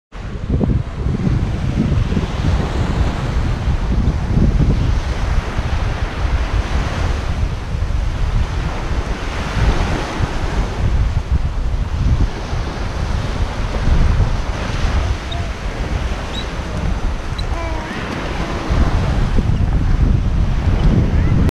Tutorial Bebè carretto spiaggia
Trasporto di Bebè & cose utili in spiaggia con carretto equipaggiato di pneumatici da sabbia.
Costruito in lega leggera su smonta per essere riposto nel bagagliaio dell’auto